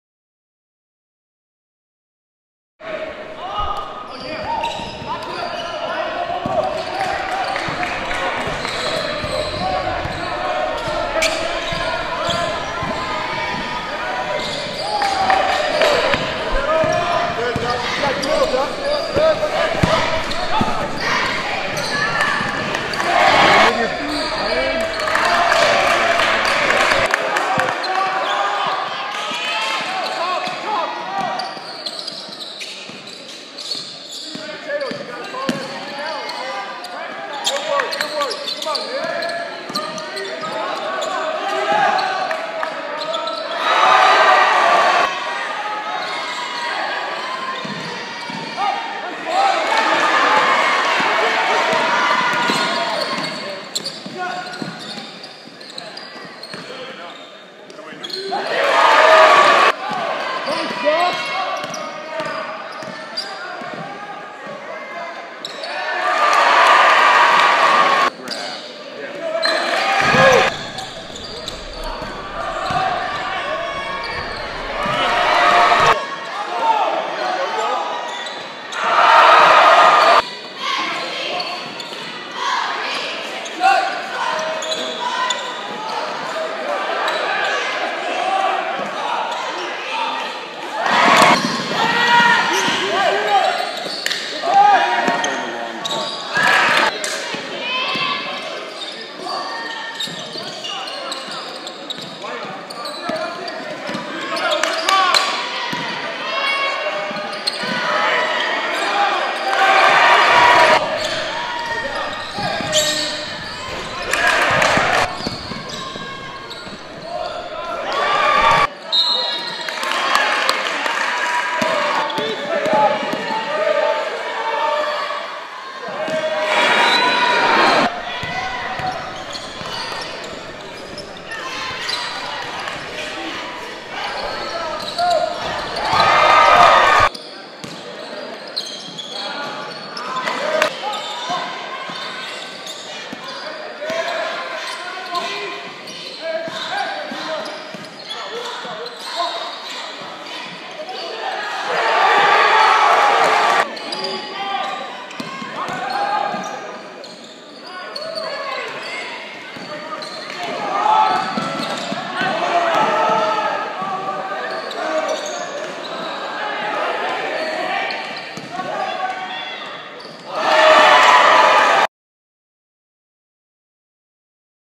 Nipomo tops Templeton in boys basketball
Four players scored in double figures, and the Nipomo High boys basketball team moved to a program-best 4-1 in Los Padres League play with an 86-59 win over Templeton on Friday in Nipomo. Video by Ale